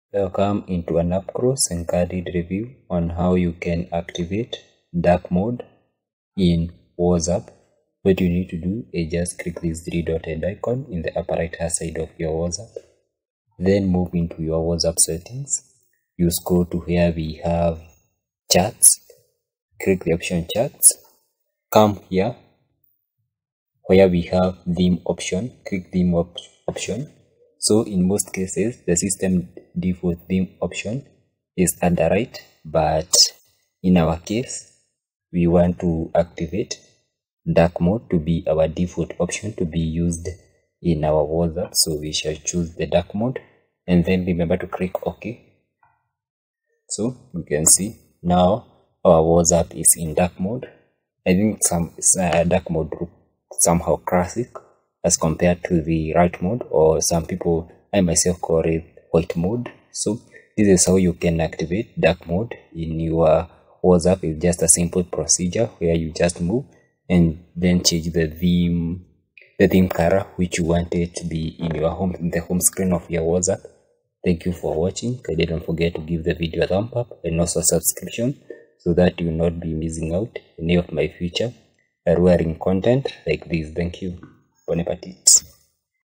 WhatsApp Dark Mode Kaise Kare? Enable Super Dark Mode On WhatsApp
Turn On Dark Mode WhatsApp||Dark Theme WhatsApp Settings
In this video I will show you how to turn on dark mode settings on WhatsApp-Dark theme WhatsApp mode